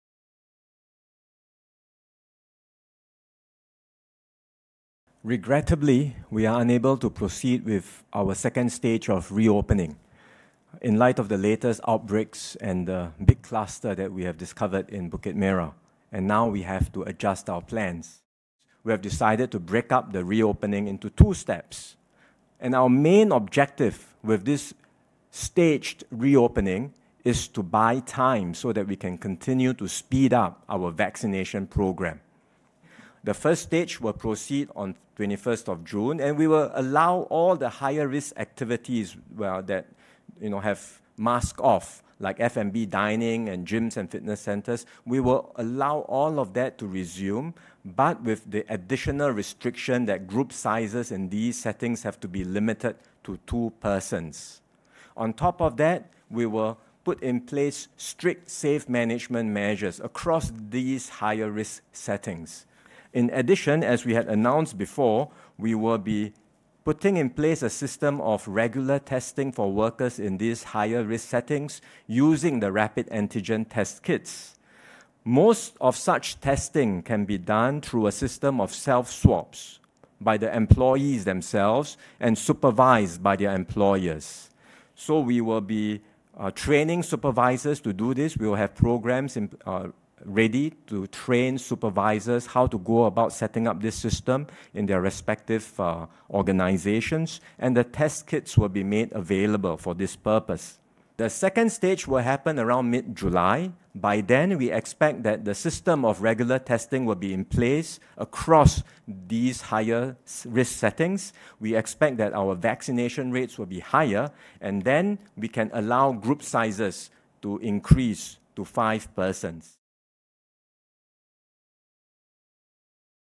Two-stage phased reopening to Phase 3A (Heightened Alert)
Singapore’s further easing of tightened measures will now occur in two phases, in light of persistent COVID-19 community cases in Singapore:

From 21 June, F&B dine-in and indoor mask-off sports/exercise can resume in group sizes of up to 2 persons.

From mid-July, the group sizes of such activities will increase to a maximum of  5 persons.

Regular testing for staff in higher risk settings will be mandatory from mid-Jul.

Minister for Finance Lawrence Wong was speaking at the Multi-Ministry Taskforce press conference on 18 June 2021.

Video: Mediacorp